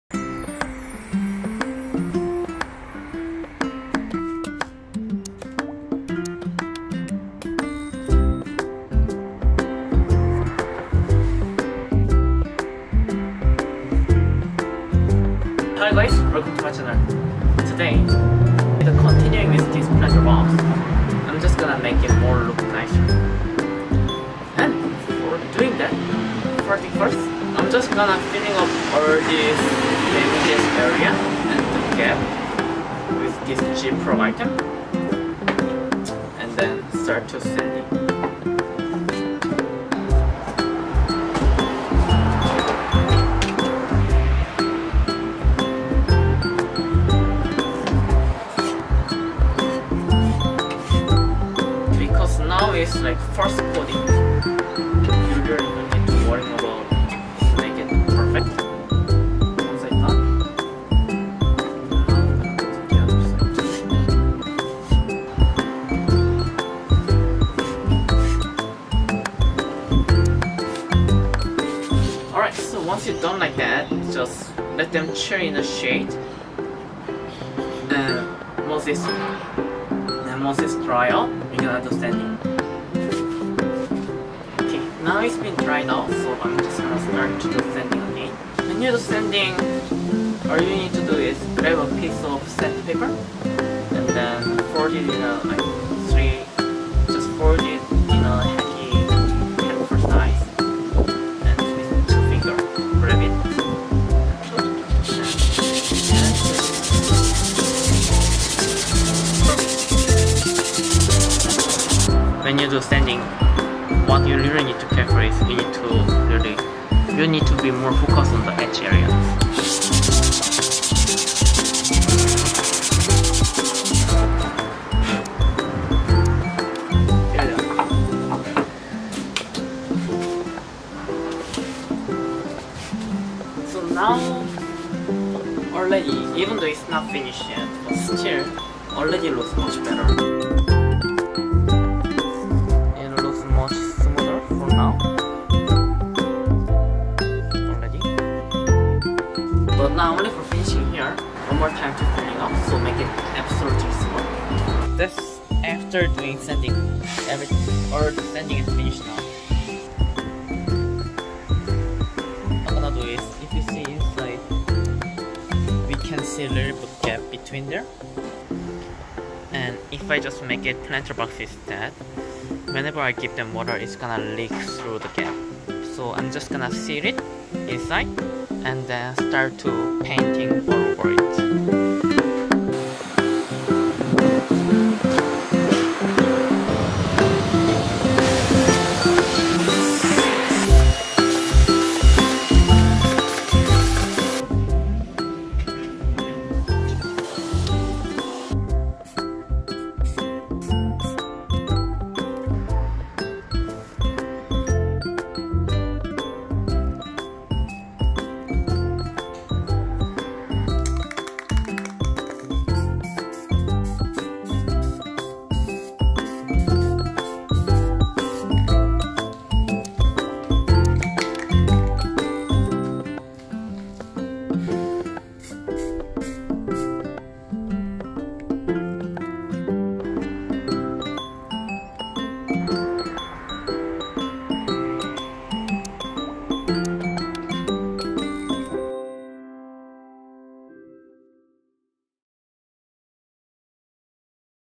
How to make modern style planter box DIY | make your Wooden planter box look nicer
Hi, everyone..!
this is favourgreen

last week, I uploaded how I make Wooden box in simple steps

and continuing the work, this time I made that planter box looks more modern style with sanding, painting, coating.
hope the time video is helpful and thank you

favourgreen